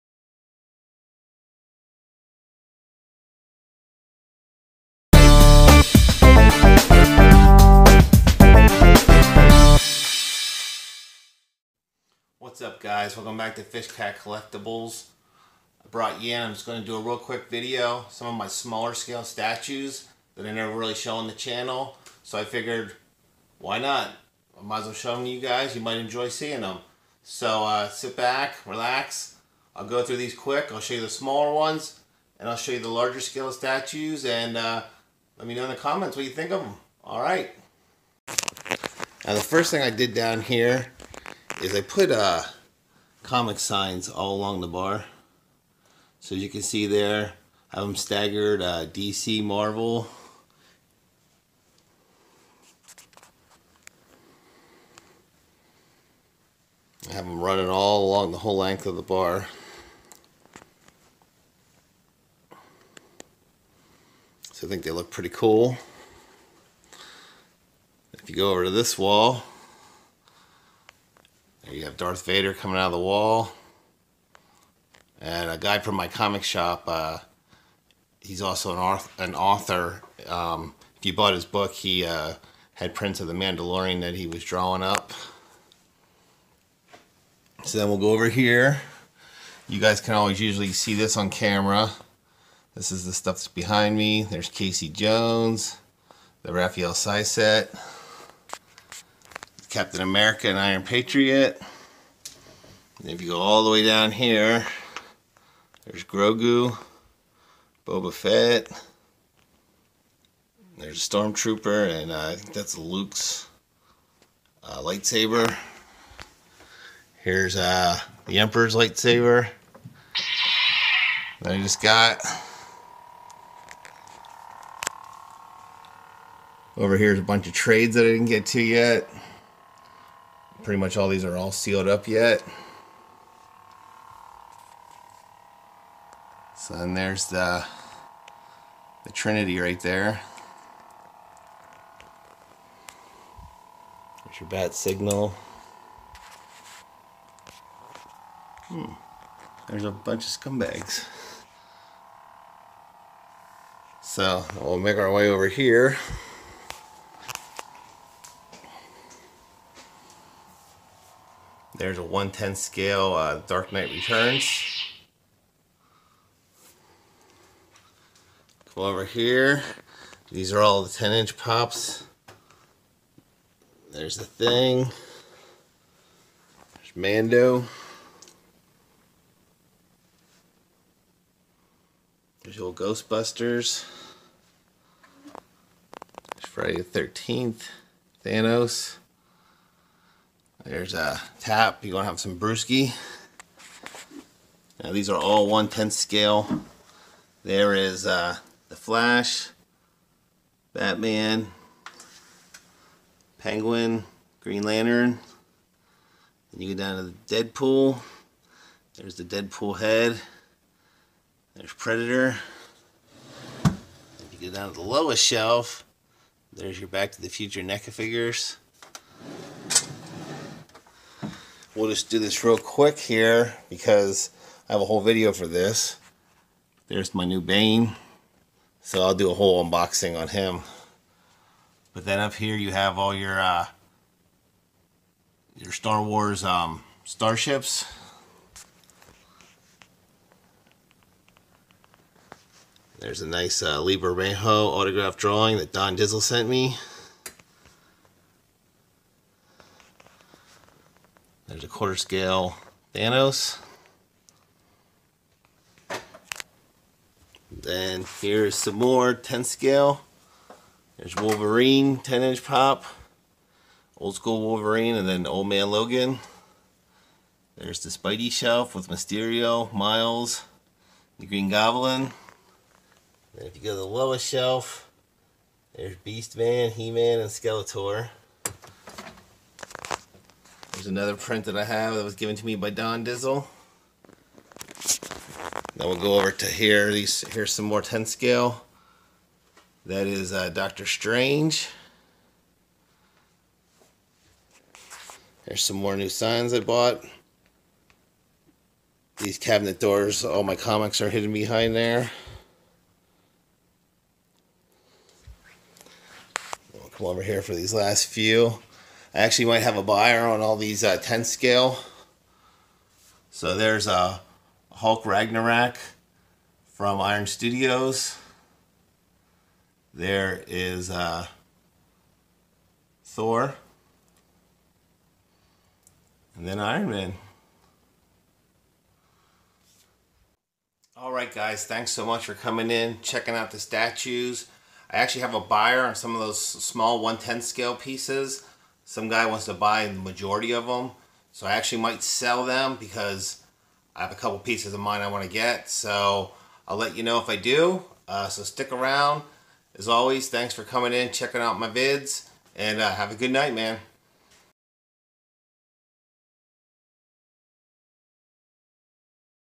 Mancave Statues and Funko tour
Thanks for checking in and seeing some of my smaller scale statues and funkos.